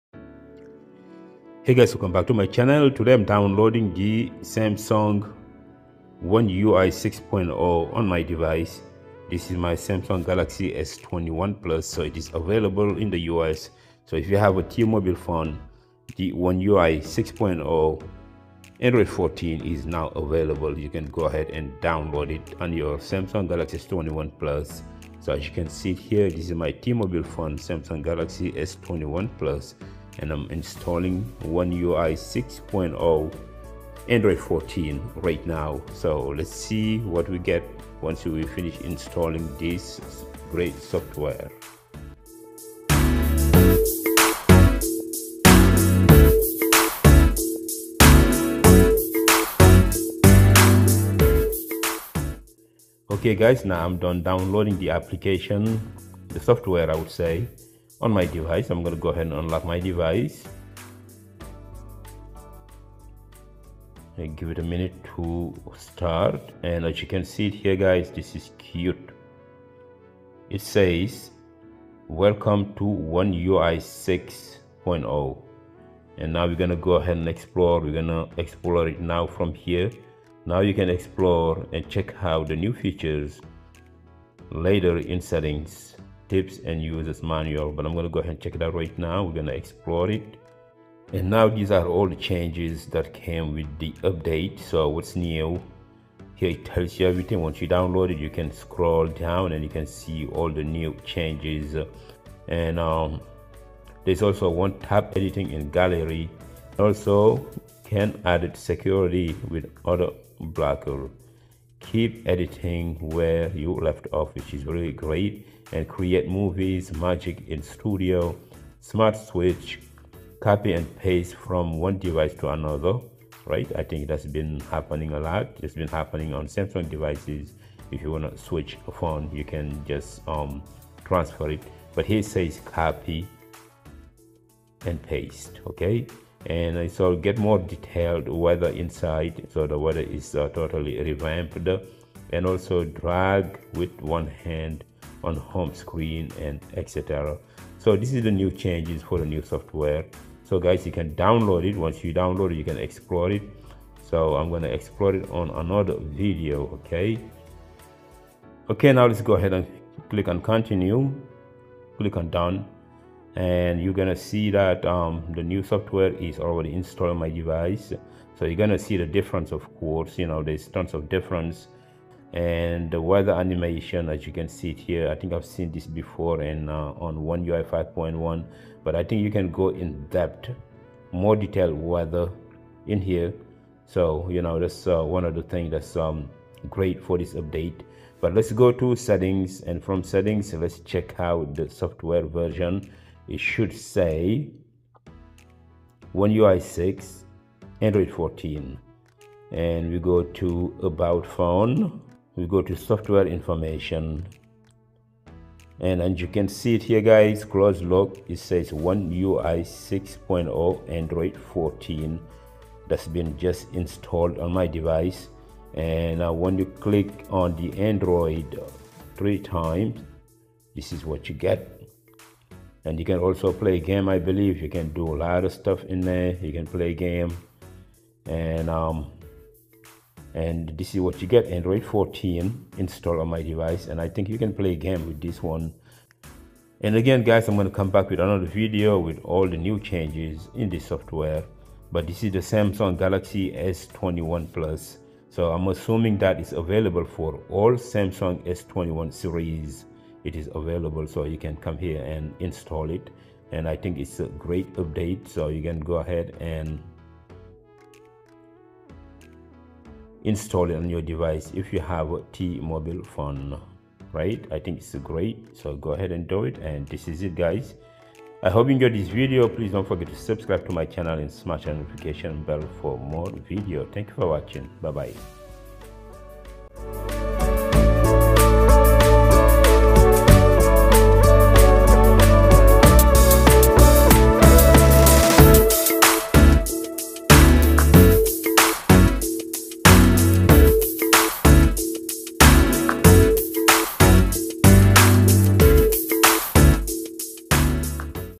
Official One UI 6.0 & Android 14 on Samsung Galaxy S21, S21 plus, S21 ultra - ASMR
Download and Install One UI 6.0 (Android 14) on your Samsung Galaxy S21

The official One UI 6.0 (Android 14) update is available for all Galaxy S21 models (S21, S21+, and S21 Ultra) as of December 1st, 2023.

There are two ways to download and install the update:

1. Over-the-air (OTA):

This is the easiest and most recommended method. To check for the update:

Go to Settings, Software update, Download and install.

Download and install option in Software update menu

If an update is available, it will be downloaded automatically. Once downloaded, tap Install now.

Your phone will reboot and the update will be installed.

2. Smart Switch:

You can also use the Smart Switch app on your computer to download and install the update.

Download and install Smart Switch on your computer.

Connect your phone to your computer using a USB cable.

Open Smart Switch and select your phone.

Click on Software Update.

If an update is available, it will be downloaded automatically. Once downloaded, click on Install.

Here are some things to keep in mind before installing the update:

Make sure your phone is charged to at least 50%.

Back up your phone data.

The update may take some time to install.

Here are some of the new features included in One UI 6.0:

A new Quick Settings layout

A media player widget with a waveform animation

A new font

New emoji designs

More font and placement options for the lock screen clock widget

Improved performance and stability

New security patches